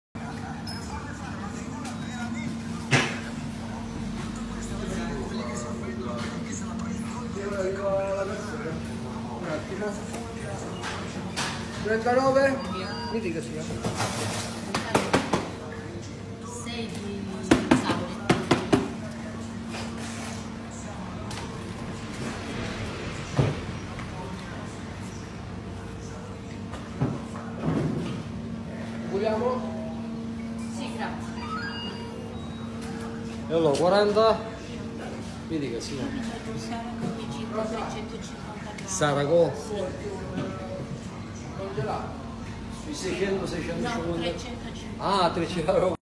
Dai nostri clienti: Red Fish Pescheria Acireale
Pesce freschissimo direttamente dalla flotta Red fish al banco della pescheria, pesce surgelato di ottima qualità e pesce pronto cuoci in Via Nazionale per Catania ad Acireale